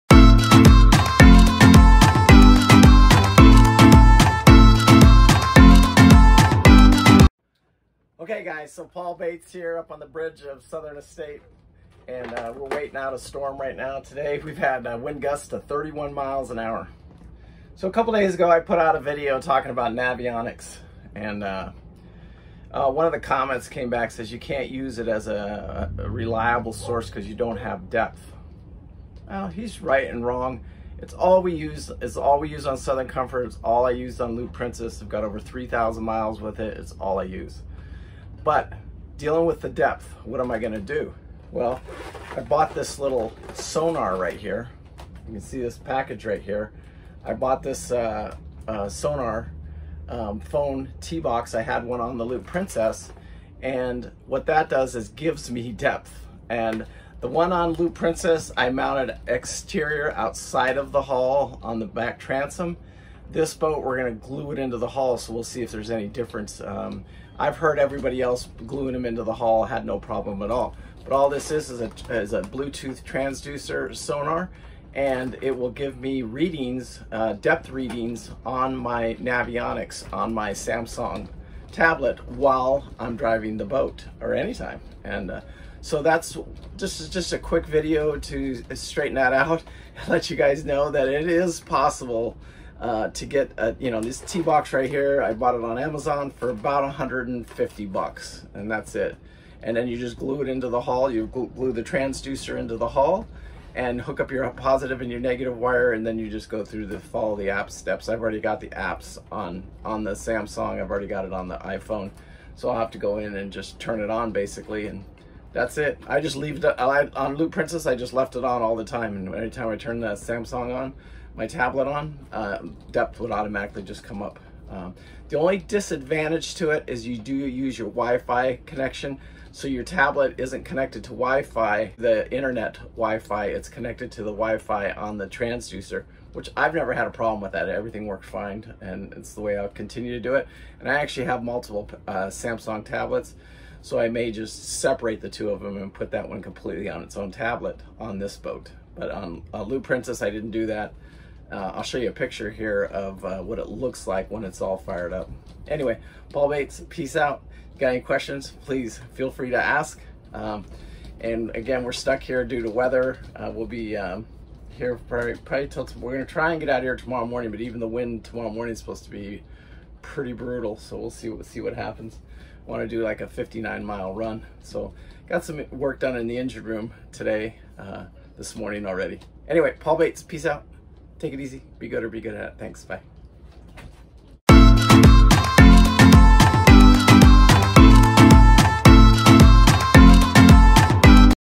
Budget Boating T box Sonar works with Navionics for Depth sounder doing America's Great Loop Vexilar
Budget trawler adding depth sounder that will work with Navionics for budget boating using only Samsung tablet and T-box Iphone sonar Bluetooth to your tablet Vexilar sonar